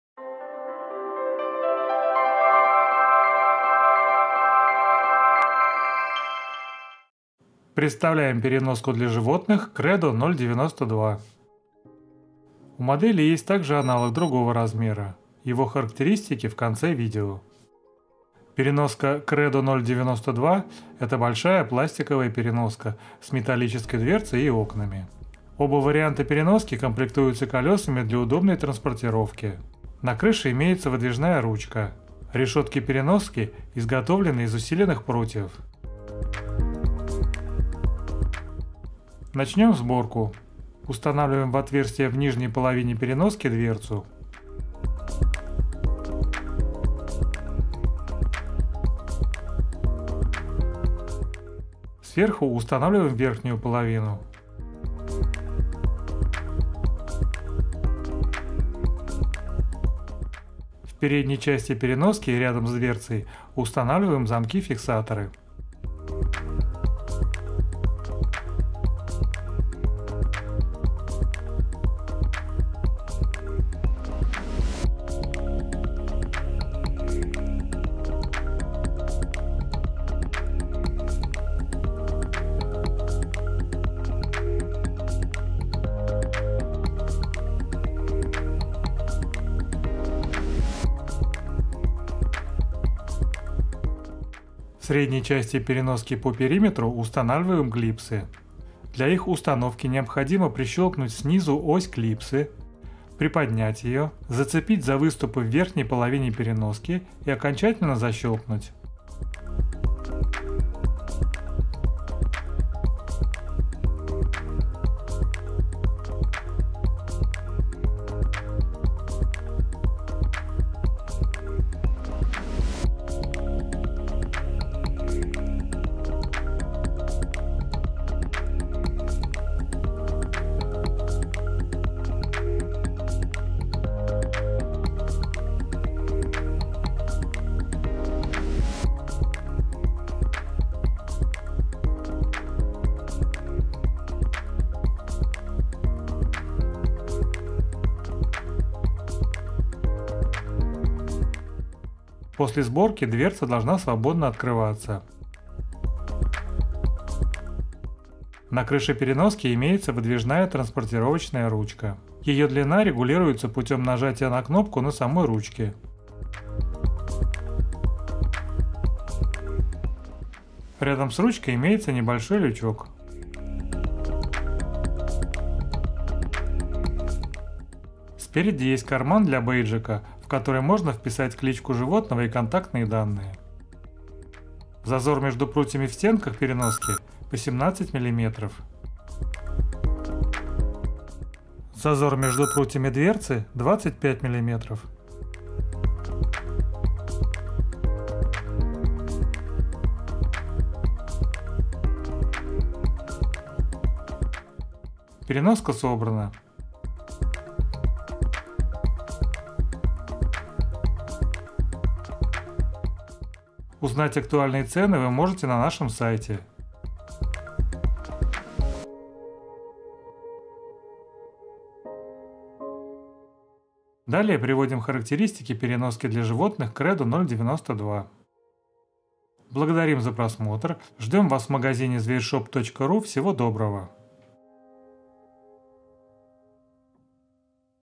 Переноска 092 для животных на колесах Kredo
Интернет-магазин товаров для животных Zvershop.ru

Переноска 092 для животных – это большая пластиковая переноска с металлической дверцей и окнами. Переноска 092 имеет колеса.

Данная модель также весьма большая. В эту переноску поместятся разные средние животные, например, кошки, маленькие породы собак.

Дверца переноски открывается с одной стороны. Эту переноску очень легко перемещать благодаря колесам и большой длинной ручке.

Переноска 092 тоже имеет карман для бейджика, в который можно вписать кличку животного и контактные данные.

Цвет переноски 092: желто-коричневый.

Размер переноски: 70*48*61 см, ее вес: 7,3 кг.

У этой модели есть аналог большего размера - переноска 093: 78*53*72 см. Вес: 7.6 кг. Она также отличается цветом, она полностью черная. Расстояние между прутьями- 2,85 см.

Расстояние между прутьями переноски 092 - 2,5 см на дверце и 1,8 см на стене.

Производитель: Kredo, Китай.